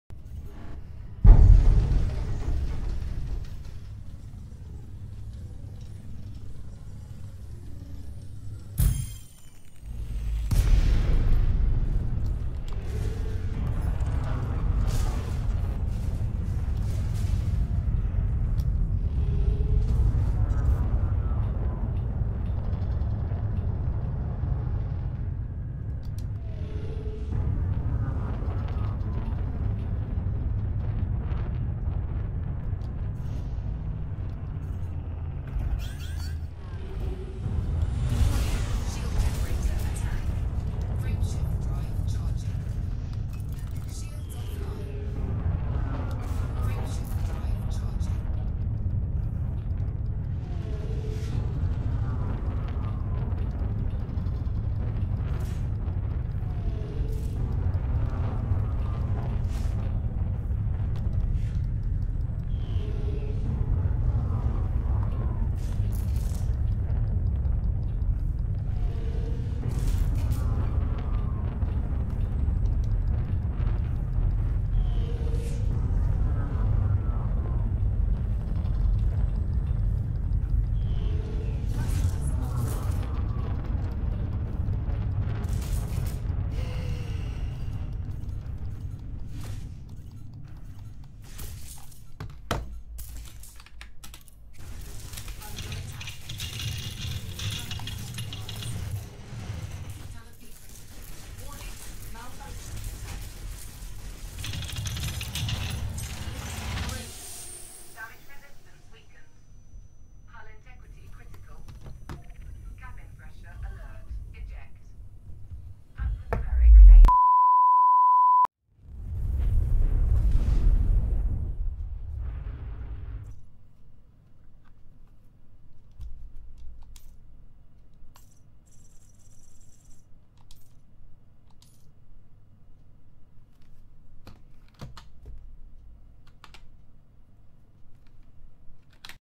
Federal Corvette losing its shields in an instant! Reverbirating Cascade Torpedoes
Nerf on shields too stronk pls fix

Basically what happened is: Torpedoes with Reverbirating Cascade upgrade that overcharged my shields resulting to my shield generator blowing up :P